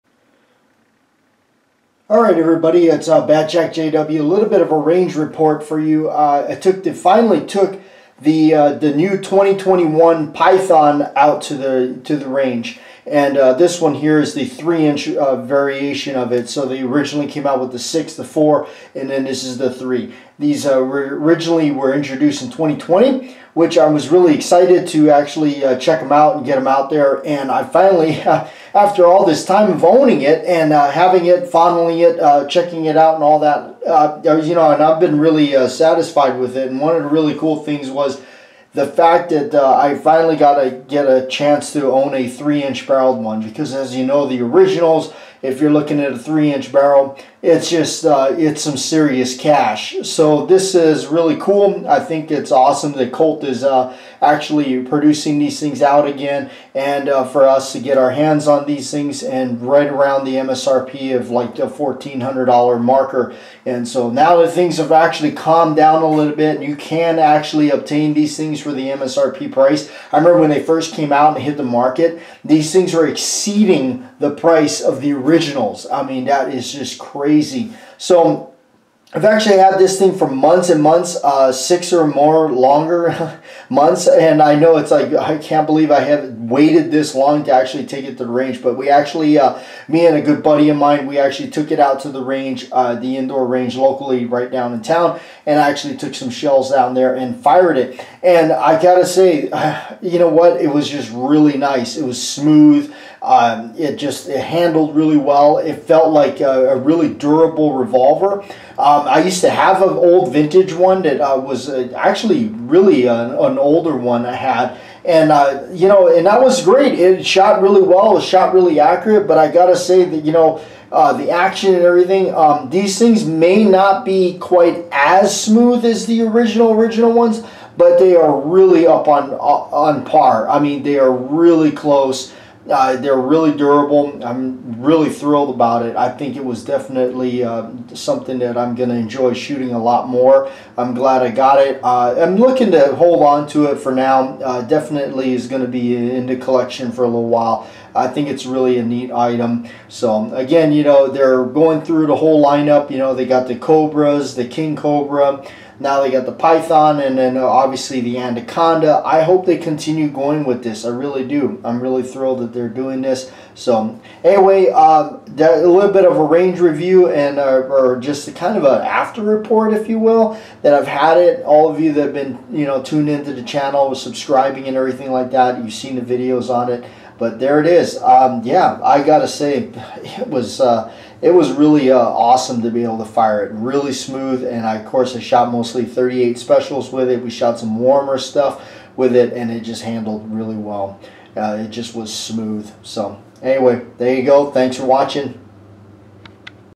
Did this thing work?
A overview of the new 3 inch barrel Colt python 357 magnum stainless steel.

C B productions Barnaby the Roo

Associated film
I F T & i got a fokx

fc j622